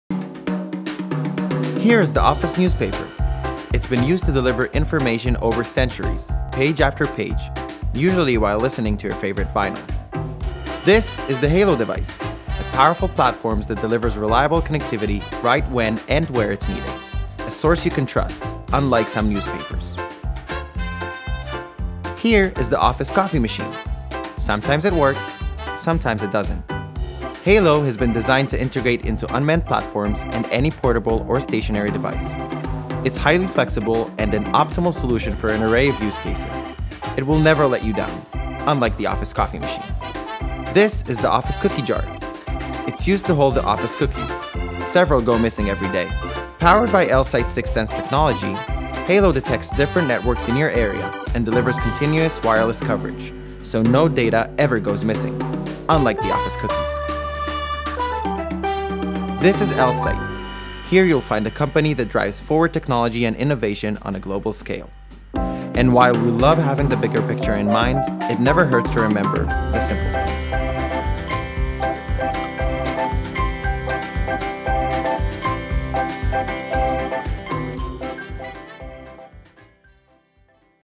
Elsight - Newspapers, Coffee Machines, Cookies, and Halo?..
What does the Halo connectivity device have in common with the 3 mentioned above?  Well, if you are looking for a reliable source, something that continuously functions, and can be integrated in unmanned platforms and many other devices, watch and discover what Halo DOES have and the newspaper, coffee machine and cookies don't!

Watch The Video Now (preferable with some cookies)!